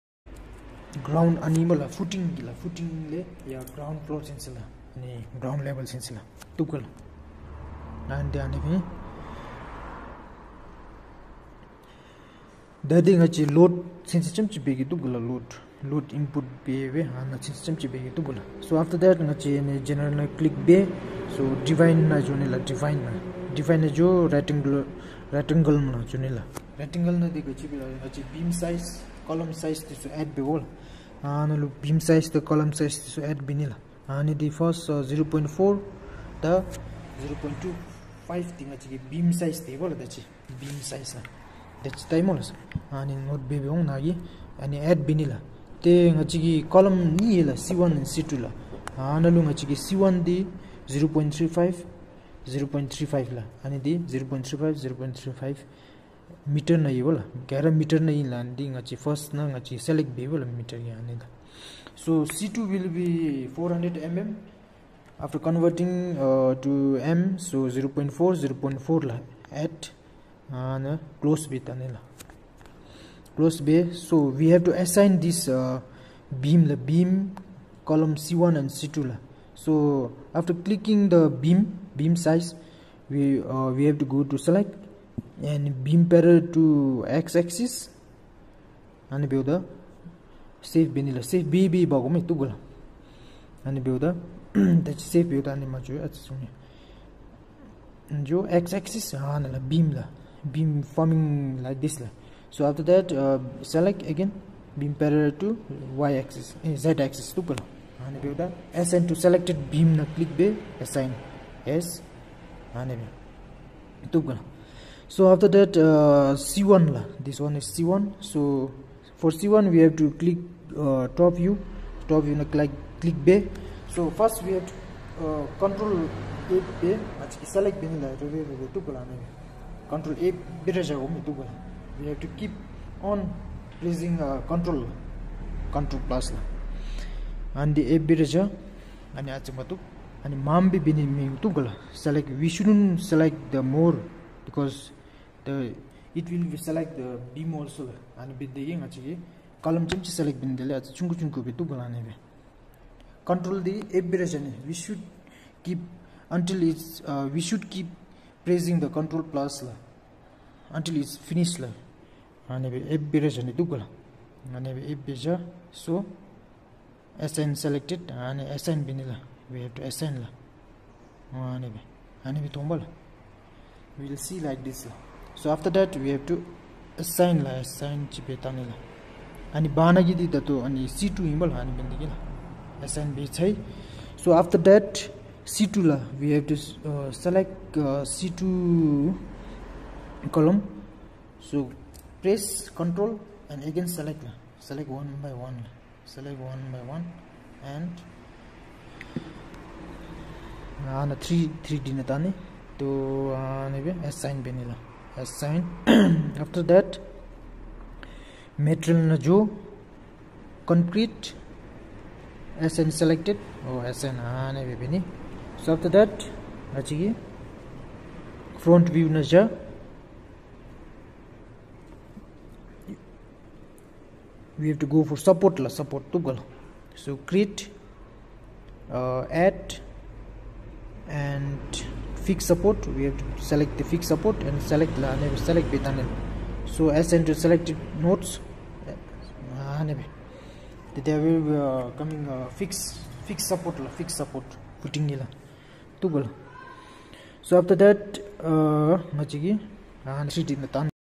Part 2. How to assign column size, beam size, support and Concrete on frame structure?
Video is continuation of Part 1.